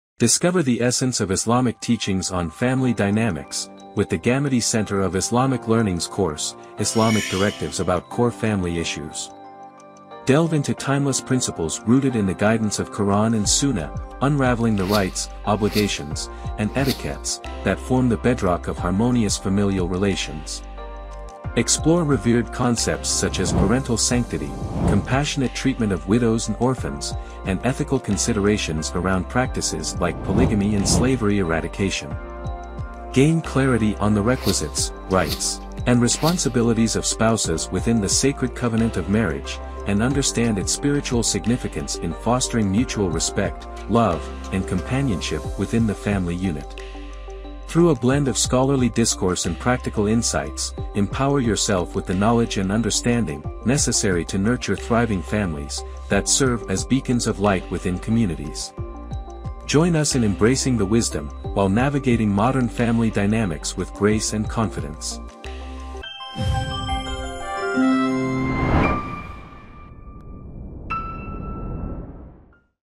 Islamic Directives About Core Family & Marriage Related Issues | An Educational Course by GCIL
Unlock the essence of Islamic teachings on family dynamics with GCIL’s course, "Islamic Directives about Core Family Issues." Explore the Quran and Sunnah’s guidance on family rights, obligations, and etiquette. Understand parental sanctity, compassionate treatment of widows and orphans, and inquire into questions about the ethics of polygamy and slavery. Learn about the rights and responsibilities of spouses within marriage, which engender mutual respect and love. Join us to gain practical insights and scholarly wisdom for nurturing harmonious families in today’s world.